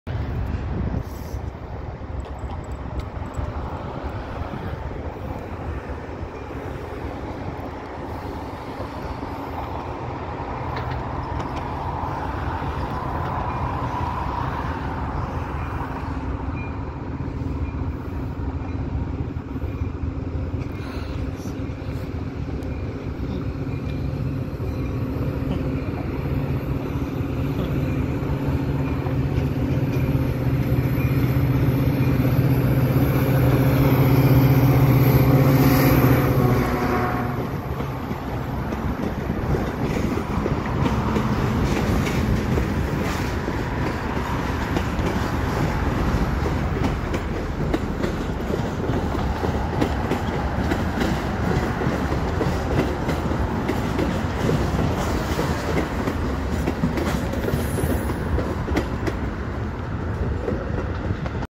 PART THREE Catching Twotenny 8/30/21
This footage was filmed on Monday August 30th, 2021

My life's precious unlikely completing puzzle piece, locomotive METX 210, heads out with its train for the final time during the PM rush period.